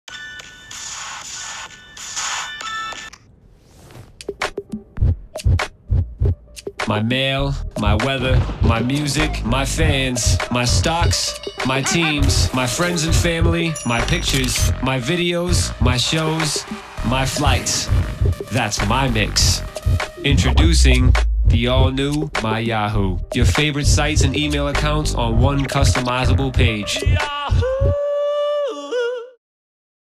The New My Yahoo feat. DJ Clinton Sparks
Check out the new My Yahoo, a customizable page that lets you access your favorite sites and email accounts all in one place!

My Yahoo's sleek layout gives you quick and easy access to what you care about most; whether its your email, calendar, sports scores, weather forecast, stock portfolio, Flickr or other go-to sites.

Choose a cool background theme to make your page even more beautiful and personal.